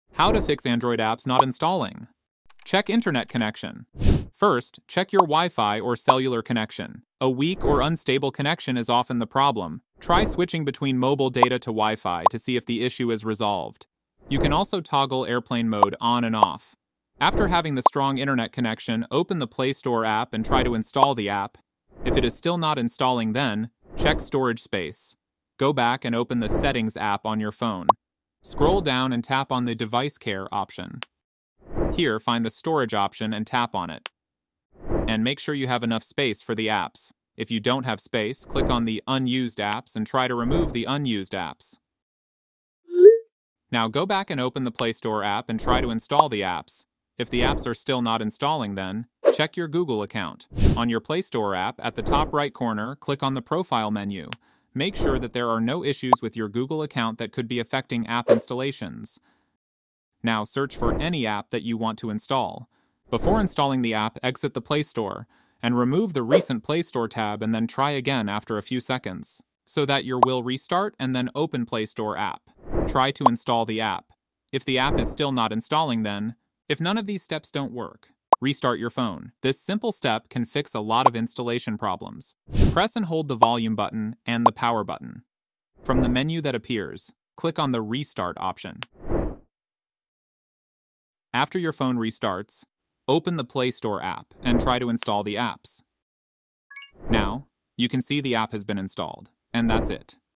How To FIX Android Apps Not Installing?
Are you struggling with apps not installing on your Android device in 2024? You're not alone! In this video, we dive deep into the common reasons behind installation failures and provide simple, step-by-step solutions to get your apps up and running smoothly. From clearing cache to checking storage space and adjusting settings, we've got you covered with the latest troubleshooting tips.

Learn how to quickly fix these issues and ensure your Android experience is seamless. Whether you're facing problems with the Google Play Store or third-party apps, our comprehensive guide will help you resolve them effectively. Don't miss out on your favorite apps any longer!

🔧 Topics covered in this video:
- Common causes of app installation failures
- How to clear cache and data for the Play Store
- Checking storage space and permissions
- Updating your Android system
- Reinstalling problematic apps

👉 Watch now and take control of your Android device! Your Android experience deserves to be hassle-free!